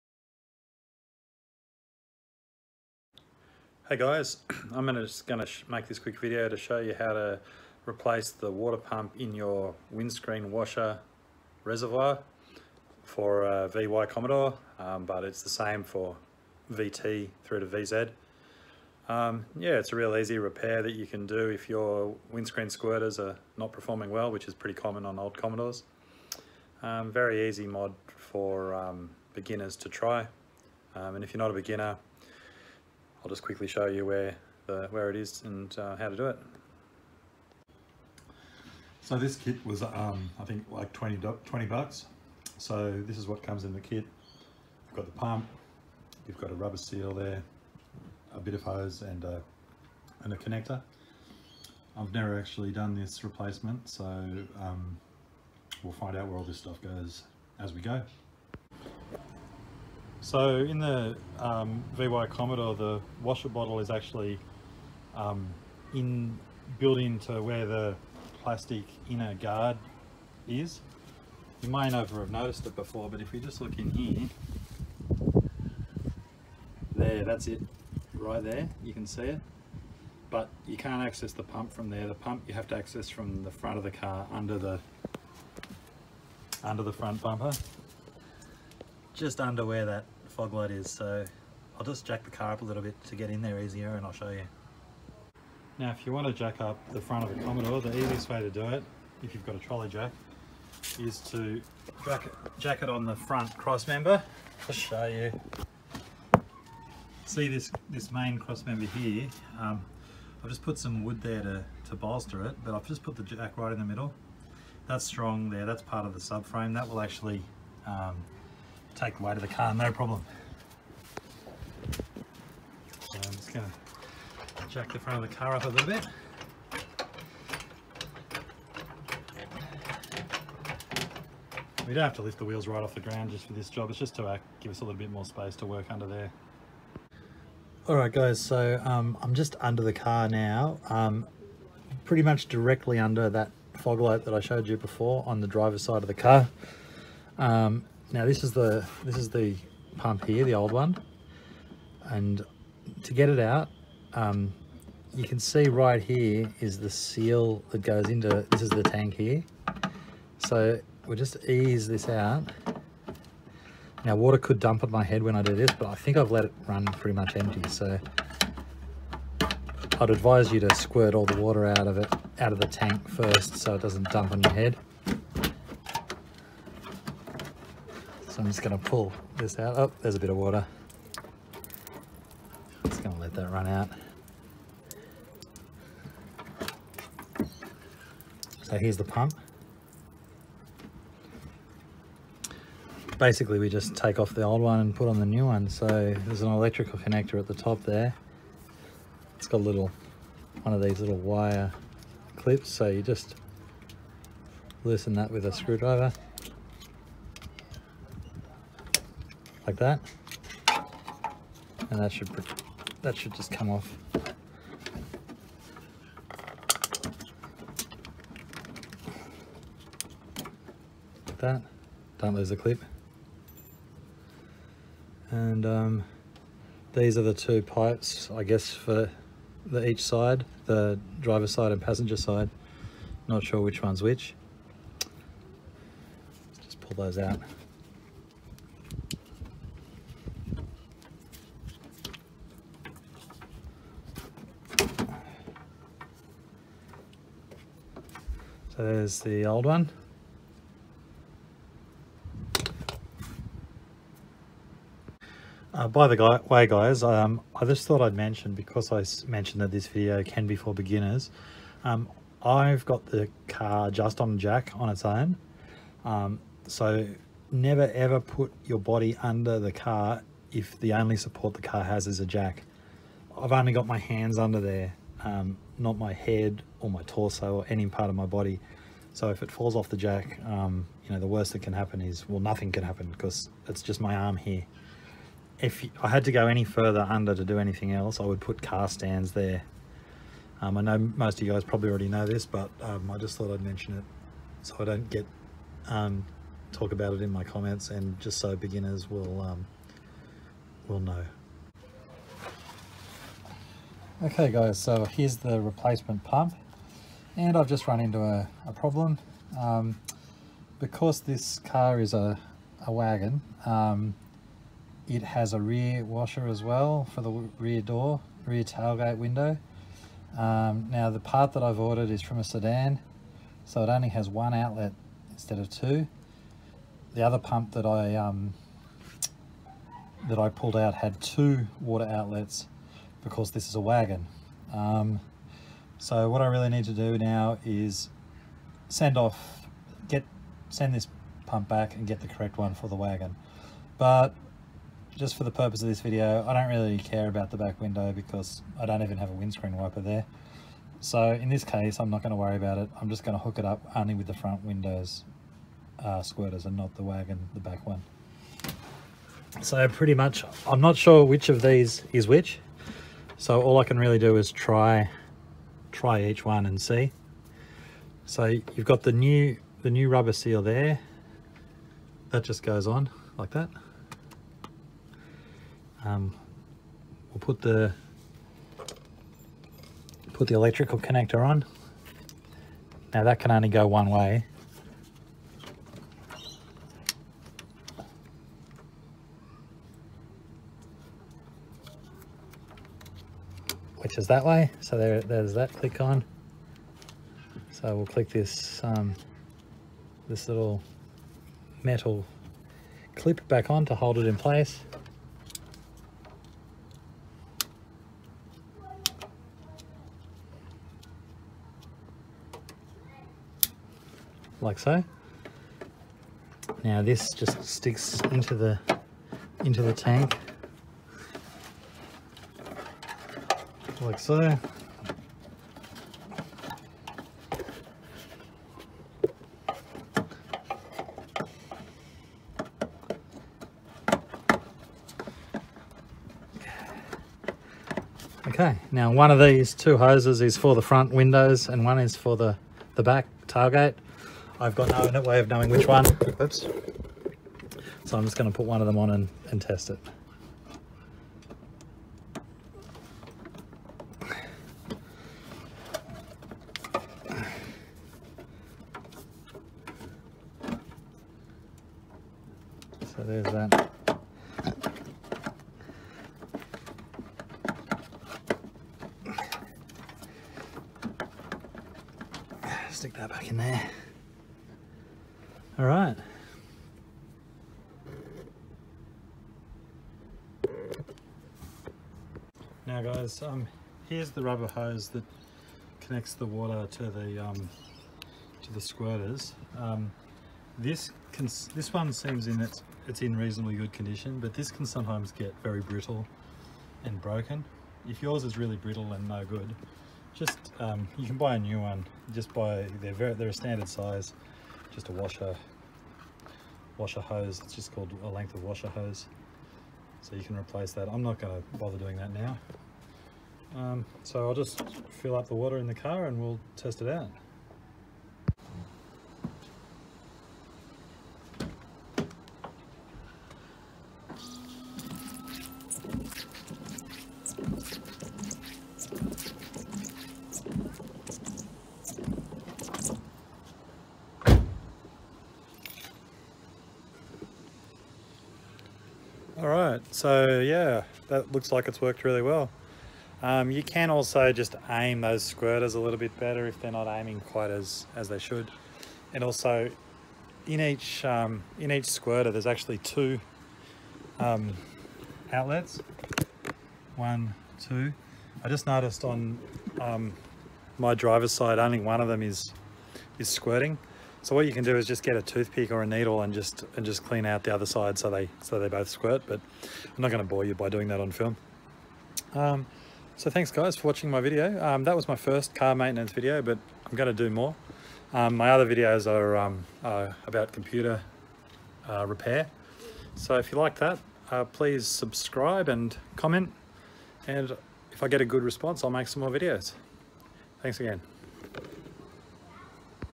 How To Replace Window Washer ( Windshield ) Pump on VT - VZ Holden Commodore 1997-2005
This is my first car maintenance video.
A very easy repair that will fix your poorly performing window washers
Great easy repair for beginners starting out learning about working on cars

2:33 Removal of old pump
6:09 Fit new pump